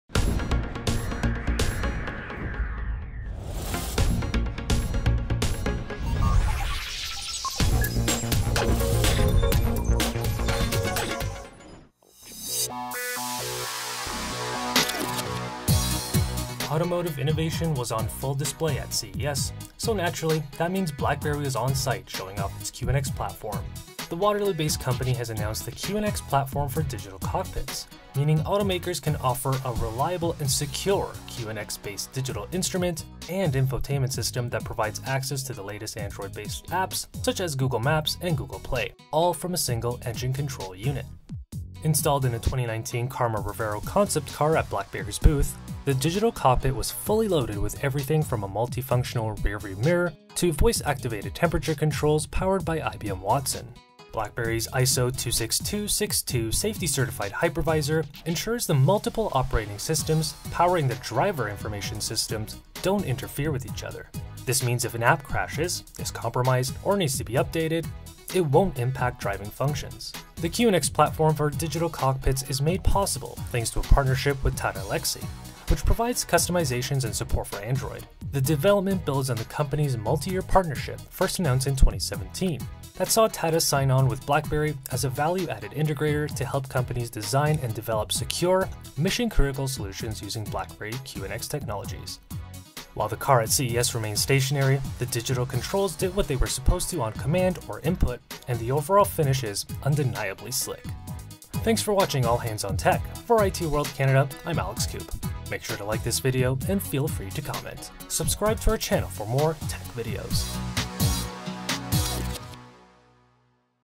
BlackBerry QNX digital cockpit platform unveiled - All Hands on Tech at CES 2019
Installed in a 2019 Karma Revero concept car at BlackBerry’s booth at CES 2019, the digital cockpit was fully loaded with everything from a multi-functional rearview mirror to voice-activated temperature control powered by IBM Watson. BlackBerry's QNX Platform for Digital Cockpits is made possible thanks to a partnership with Tata Elxsi which provides customizations and support for Android.